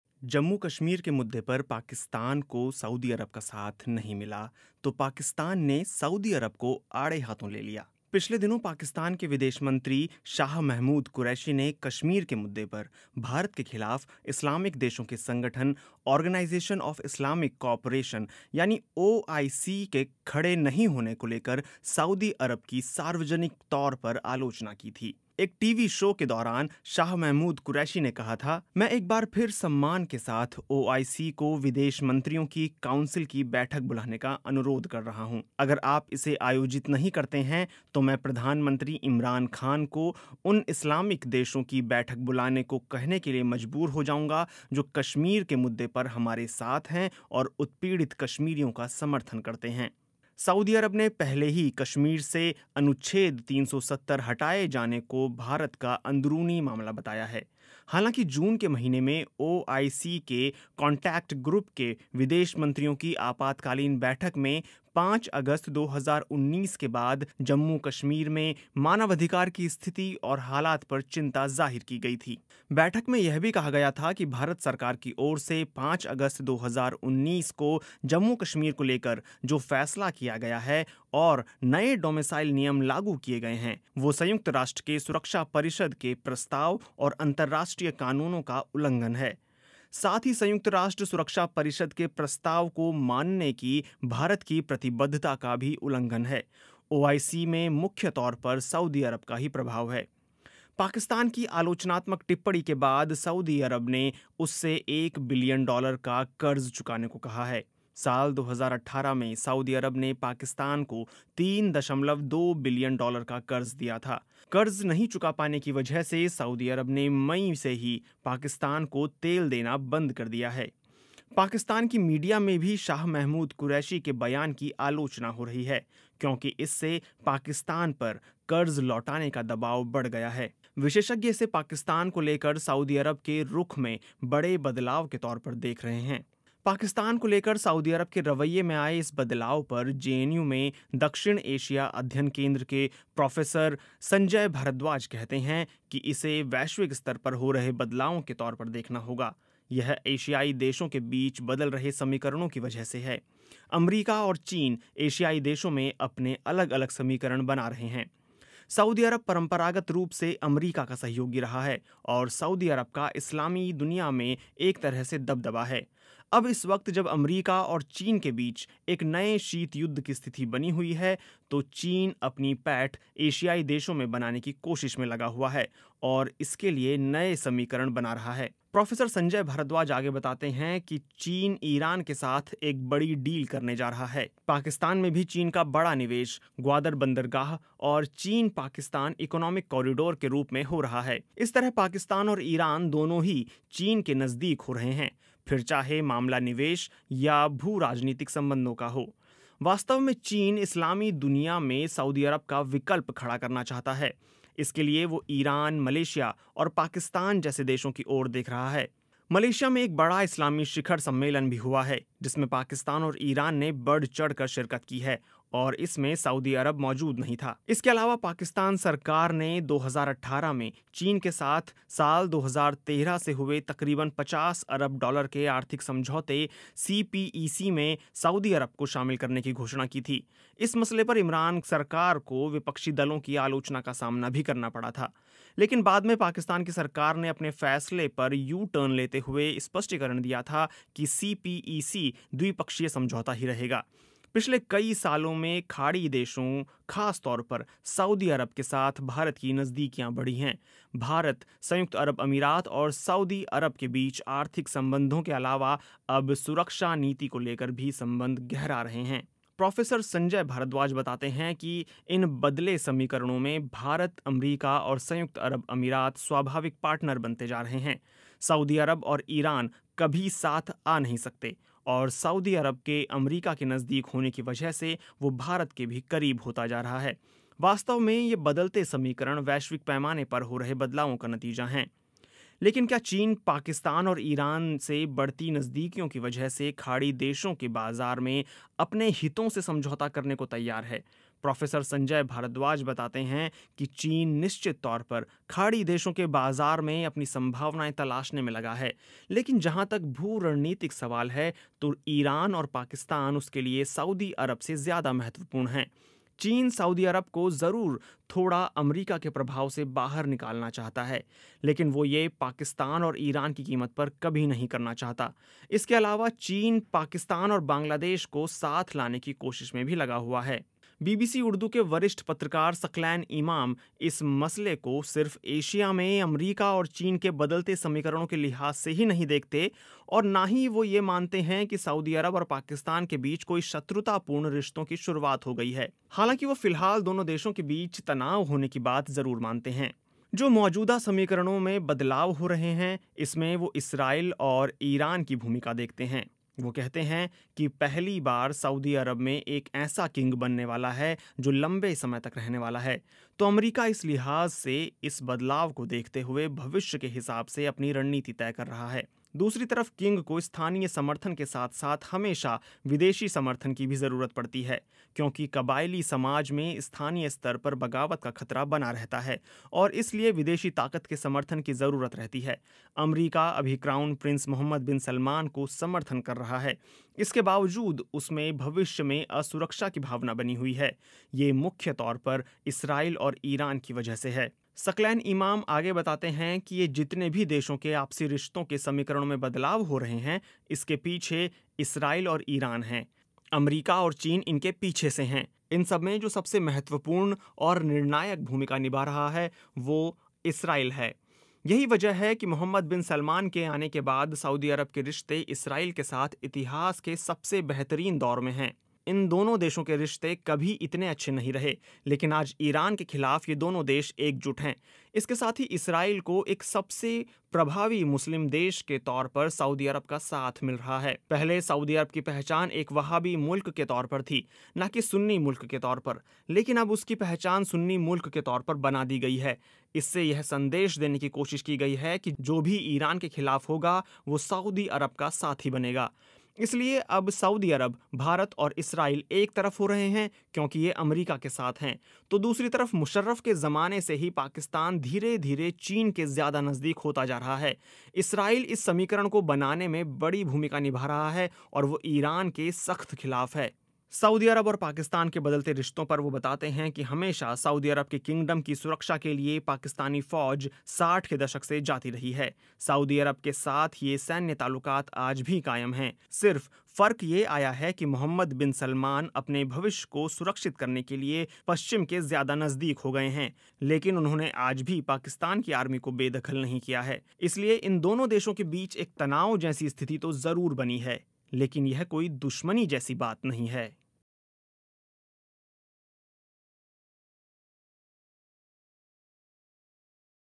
Saudi Arab से Pakistan दुश्मनी क्यों मोल ले रहा है? (BBC Hindi)
जम्मू-कश्मीर के मुद्दे पर पाकिस्तान को सऊदी अरब का साथ नहीं मिला तो पाकिस्तान ने सऊदी अरब को आड़े हाथों ले लिया. पिछले दिनों पाकिस्तान के विदेश मंत्री शाह महमूद क़ुरैशी ने कश्मीर के मुद्दे पर भारत के ख़िलाफ़ इस्लामिक देशों के संगठन ऑर्गनाइज़ेशन ऑफ़ इस्लामिक कॉपरेशन (ओआईसी) के खड़े नहीं होने को लेकर सऊदी अरब की सार्वजनिक तौर पर आलोचना की थी. सऊदी अरब ने पहले ही कश्मीर से अनुच्छेद 370 हटाए जाने को भारत का अंदरूनी मामला बताया है. पाकिस्तान की आलोचनात्मक टिप्पणी के बाद सऊदी अरब ने उससे एक बिलियन डॉलर का क़र्ज़ चुकाने को कहा है. साल 2018 में सऊदी अरब ने पाकिस्तान को 3.2 बिलियन डॉलर का क़र्ज़ दिया था. क़र्ज़ नहीं चुका पाने की वजह से सऊदी अरब ने मई से ही पाकिस्तान को तेल देना बंद कर रखा है.

स्टोरीः तारेंद्र किशोर
आवाज़ः नवीन नेगी